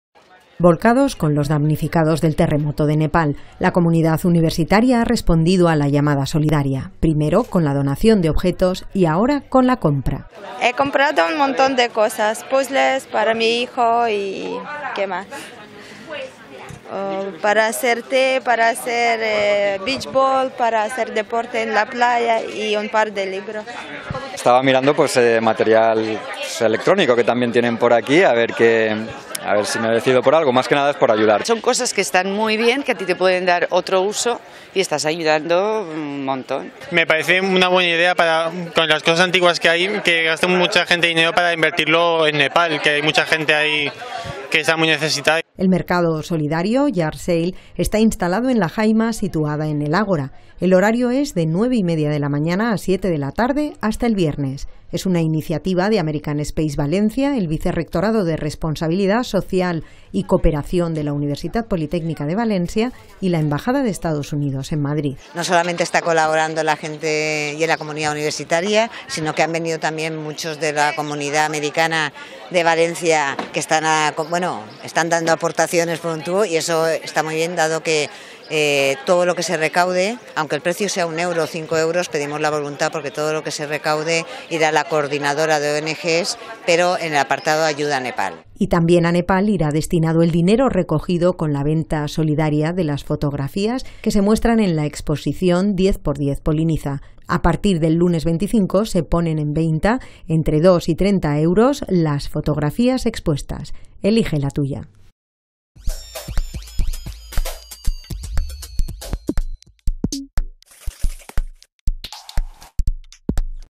Noticia @UPVTV: Yard Sale UPV y 10x10 Poliniza [20-05-2015]
Volcados con los damnificados del terremoto de Nepal. La comunidad universitaria ha respondido a la llamada solidaria. Primero con la donación de objetos y ahora con la compra.

El mercado solidario, Yard Sale, está instalado en la jaima situada en el Ágora. El horario es 9.30  de la mañana a siete de la tarde hasta el viernes. Es una iniciativa de American Space Valencia,  el Vicerrectorado de Responsabilidad Social y Cooperación de la Universitat Politècnica de València y la Embajada de Estados Unidos en Madrid.

Y también a Nepal irá destinado el dinero recogido con la venta solidaria de las fotografías que recoge la exposición “10x10 Poliniza”. A partir del lunes 25 se ponen en venta, al precio de 2 a 30 euros, las fotografías expuestas. ¡Elige la tuya!